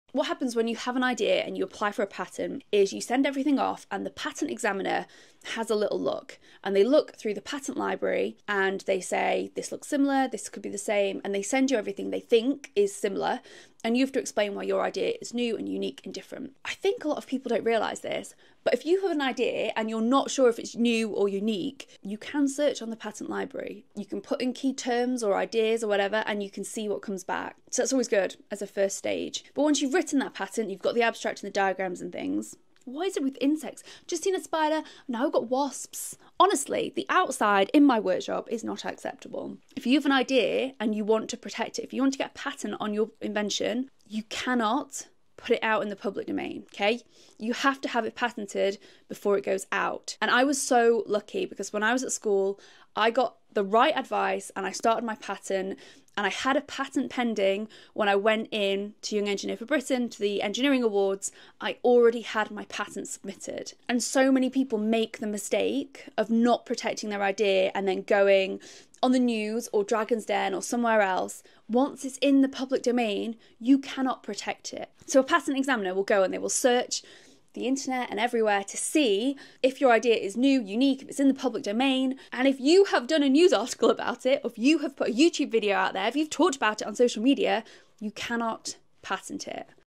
🚨 Inventors, read this before you share your idea online!
In the UK, Europe, and most of the world, any public disclosure—yes, even a school talk or an Instagram post—can kill your chances of getting a patent. 💀

Unless you’ve filed first, your idea is no longer “novel,” and that means it’s no longer protectable.

🇺🇸 The US gives you a 12-month grace period, but most countries don’t. No second chances.

🧠 Moral of the story? File before you share.